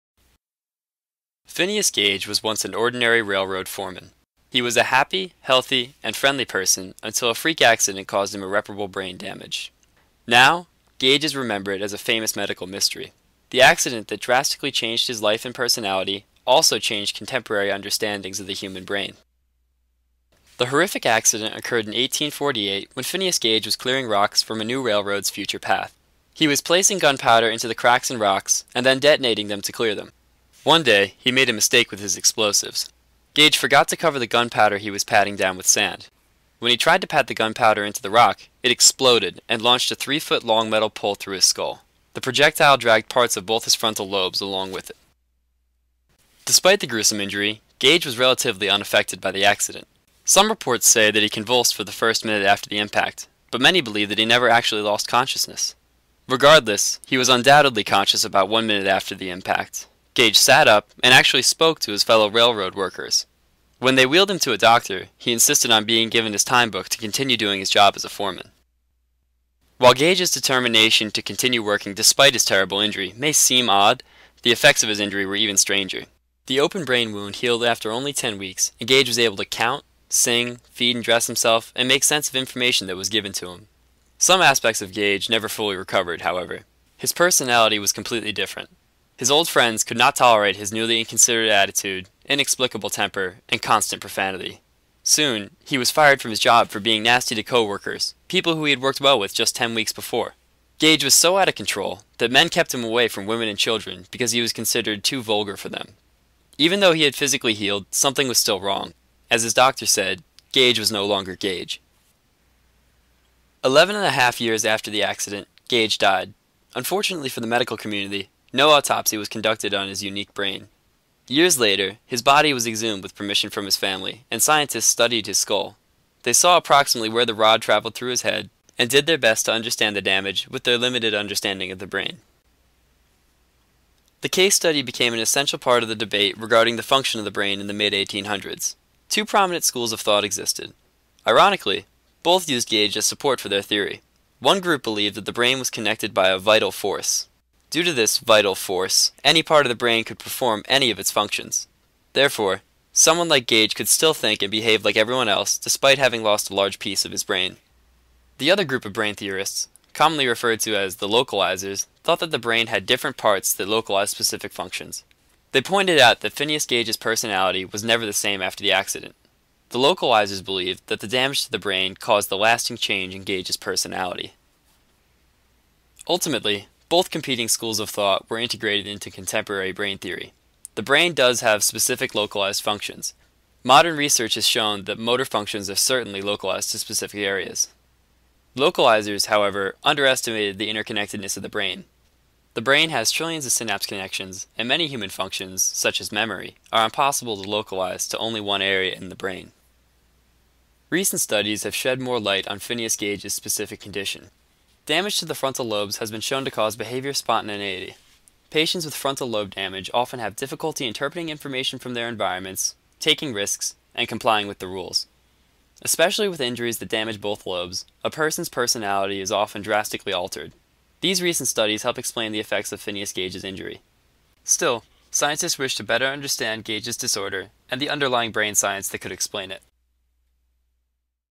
Phineas Gage Digital Story.m4v
The Story of Phineas Gage's personality change, and the underlying brain science that explains it.